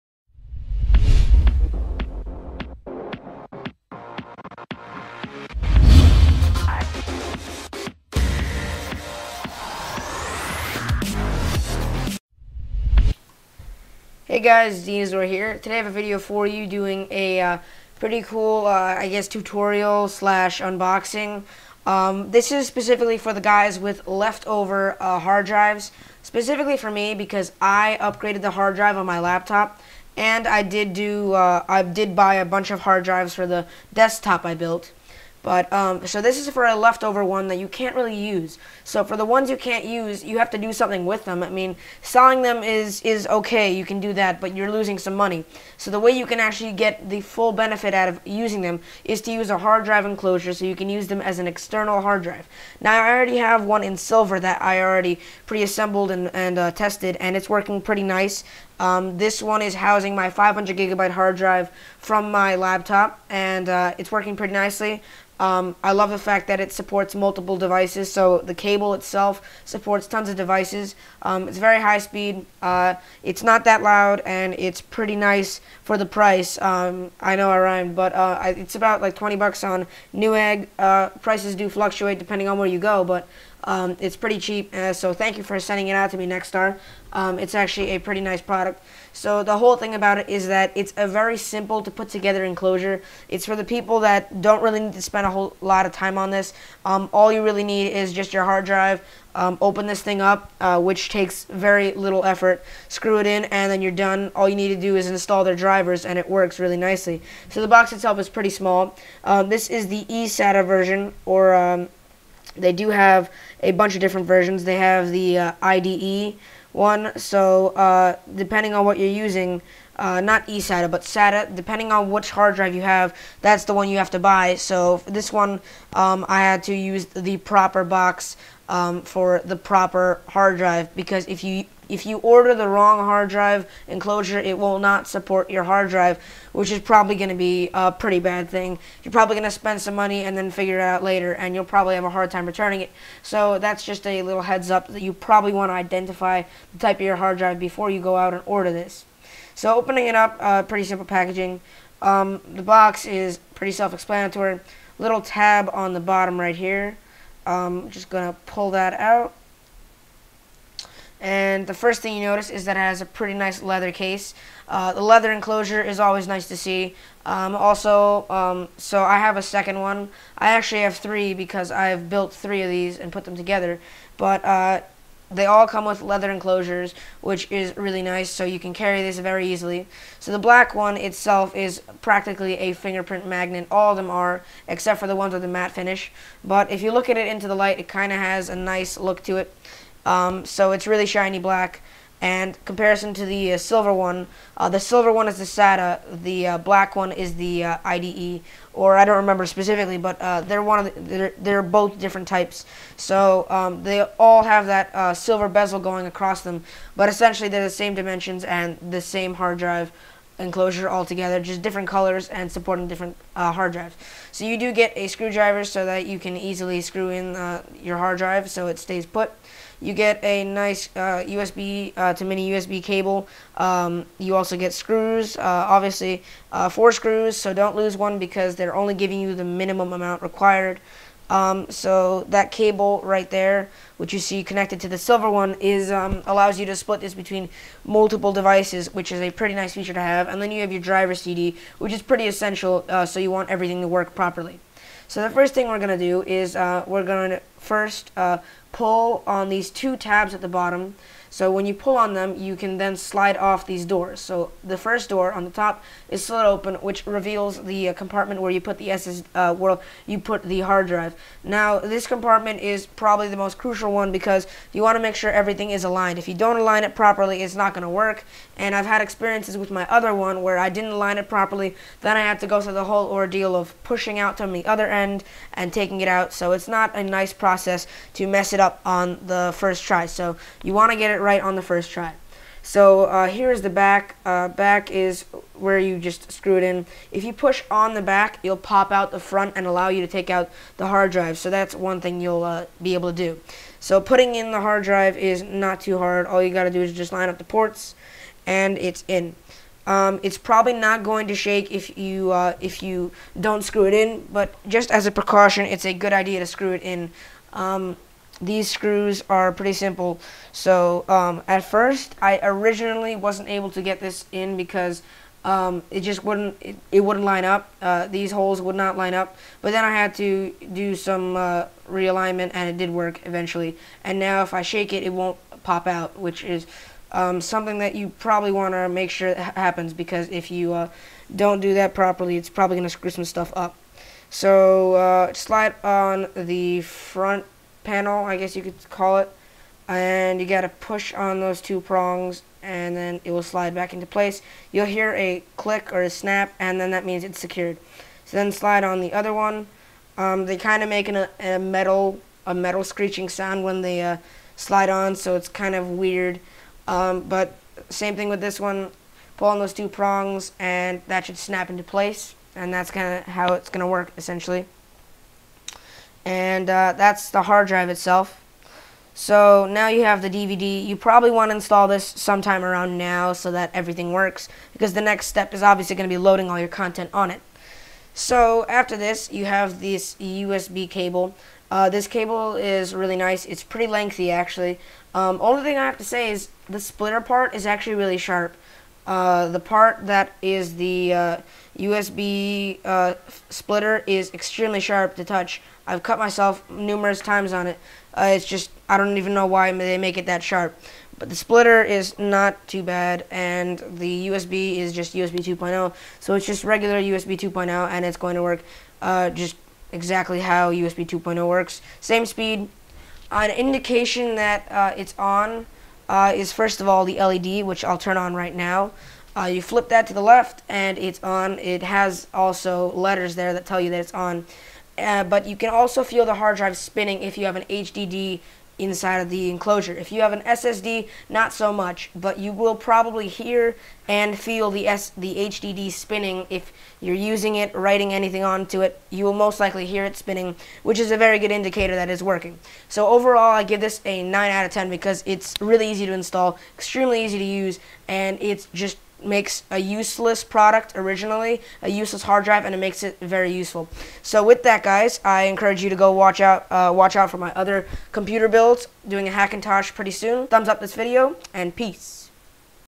How to Turn a Left Over Hard Drive into an External Hard Drive!!!
This video briefly goes over the steps, and best ways, to use a Hard Drive and turn it into an External Hard Drive. Big thank you to the guys over at NexStar for making this possible! :-)